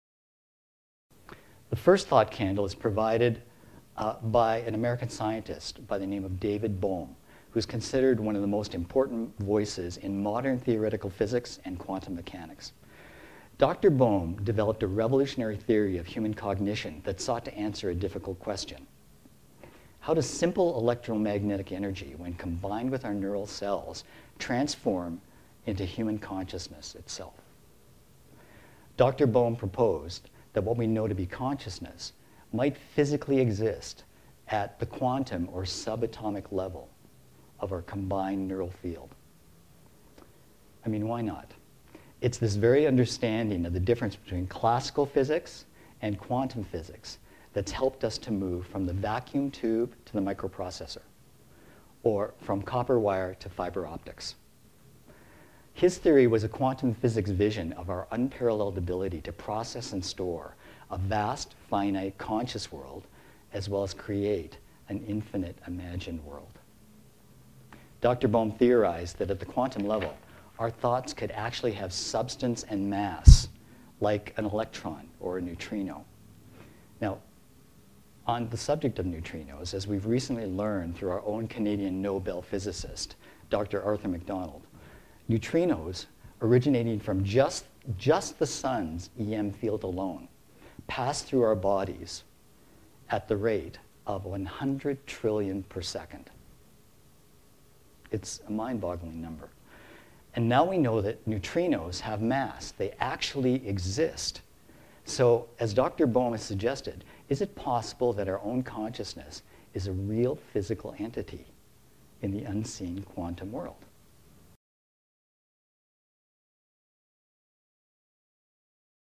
David Bohm - The Question Community
Dr. David Bohm is considered one of the most important voices in modern theoretical physics and quantum mechanics.

Dr. Bohm developed a revolutionary theory of human cognition that sought to answer a difficult question: how does simple electromagnetic energy when combined with our neural cells transform into human consciousness itself?

In this video, presenter Frederick Tamagi explores Dr. Bohm's theory and findings.